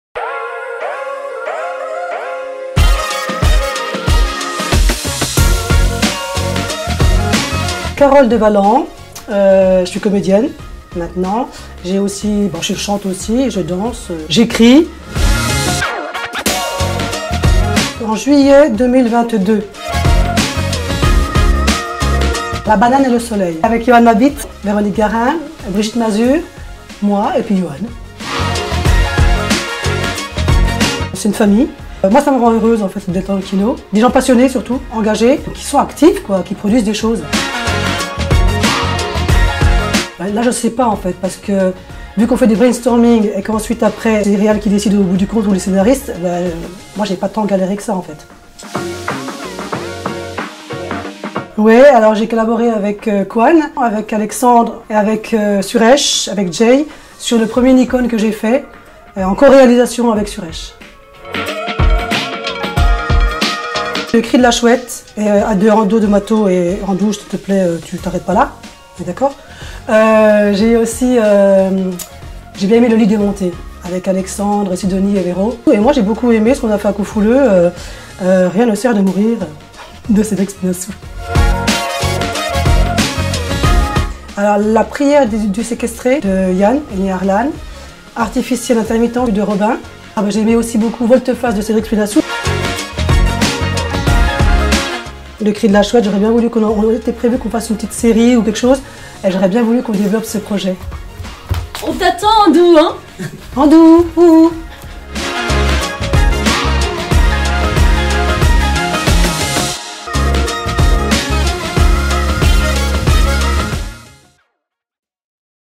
INTERVIEW EXPRESS : CAROLE DEVALLAND
Interview express des membres du kino express.
Cette fois ci: Carole Devalland
Interview réalisé par Gilles Lafayes.